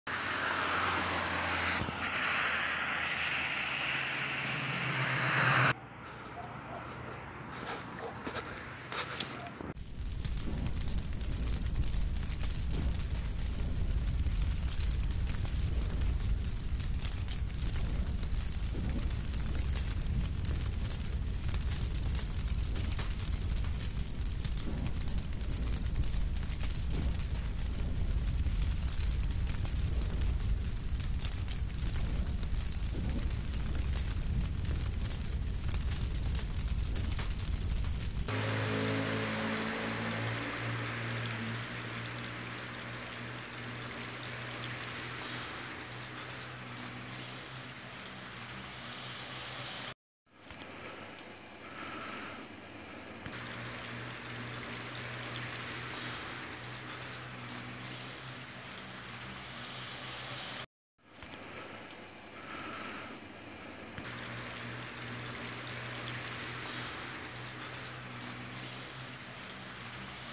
فعالیت کانون های شورشی در شهرهای همایونشهر اصفهان و کرج ۴ اسفند
انون‌های شورش و قیام در شهرهای همایونشهر اصفهان، کرج، سمنان، اصفهان و تهران با به آتش کشیدن بنرهای تبلیغاتی رژیم پا به گور و هم‌چنین به آتش کشیدن تصاویر منحوس پاسدار جنایتکار قاسم سلیمانی و از سوی دیگر با پخش تراکت و شعارنویسی برای مقاومت سازمان‌یافته مردم ایران آمادگی خود را برای فاز پایانی این رژیم قرون‌وسطایی اعلام کرده و با خون شهیدان قیام تجدید پیمان کردند.